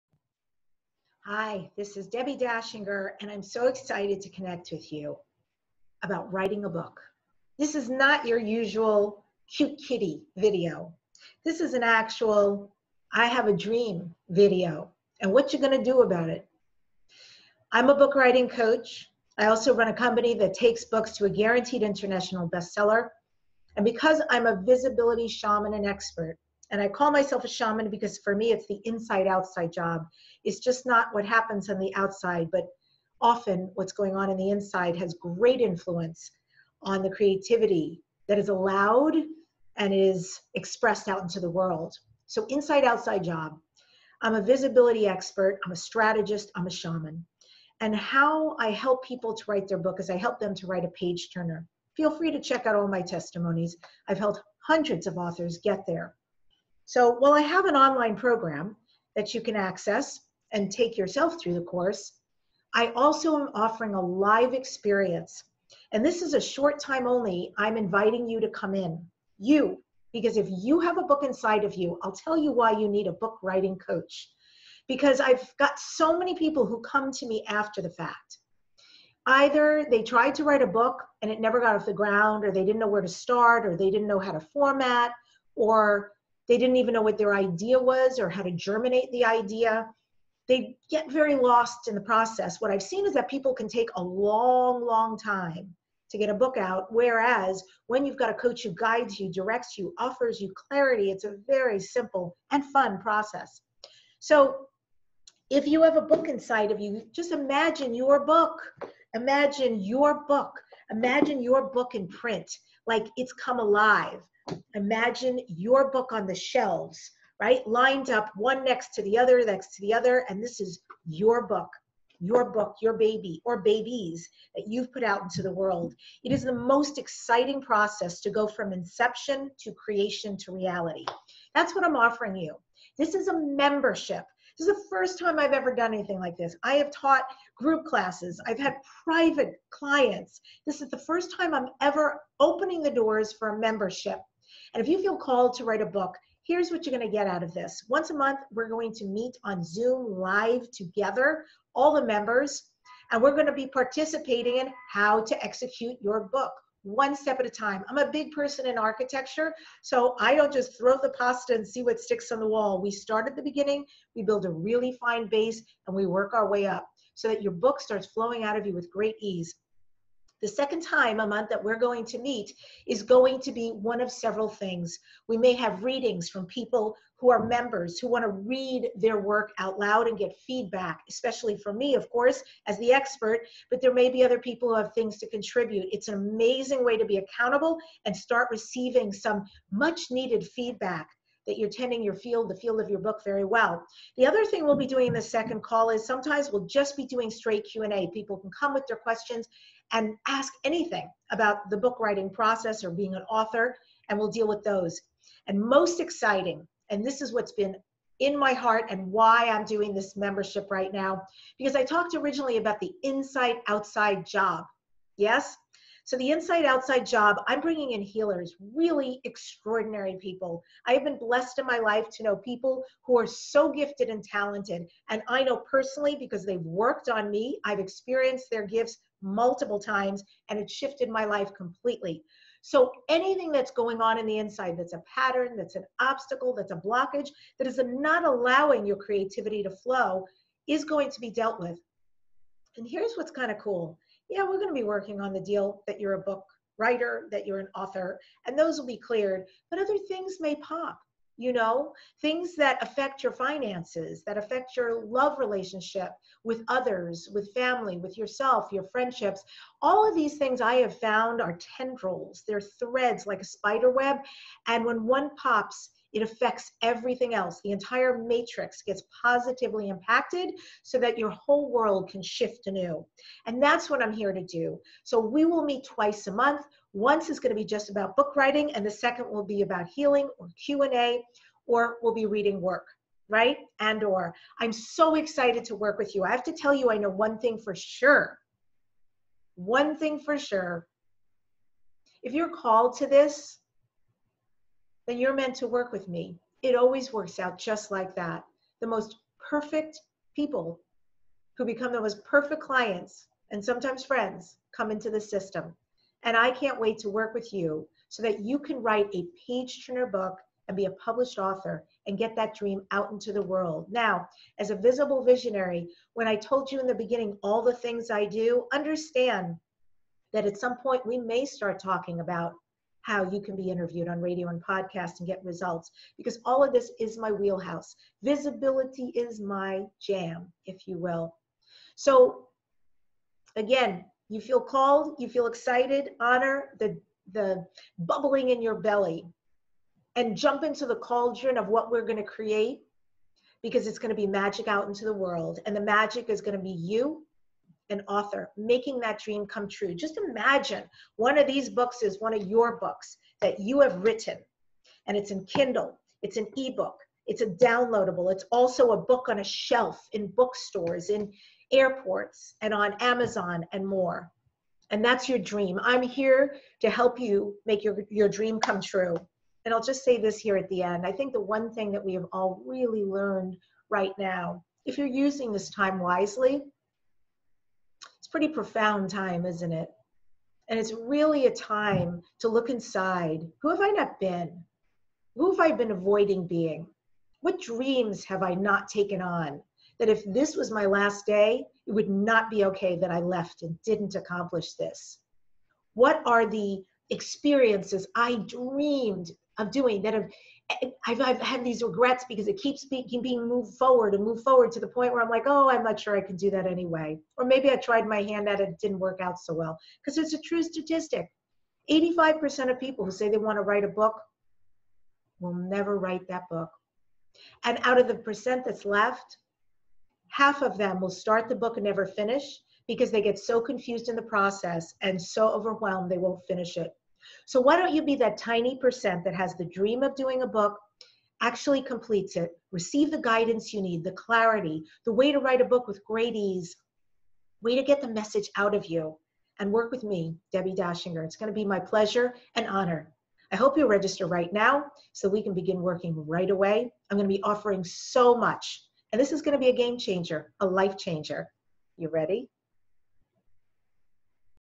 The world is at a turning point. Here's What the Virus Means. With DEBBI DACHINGER
Your Meaningful Role? The world is at a turning point. I know you can feel it. But are you living into the meaningful role you're meant to play in shaping our collective future? Or, are you feeling overwhelmed by your calling?

Are you uncomfortable by your need to become visible? It's time for you to share your vision and make the difference that only you can make. Write Your Book, Be Interviewed with Success, & Positively Change from the Inside Out While You Create Your Visibility Dream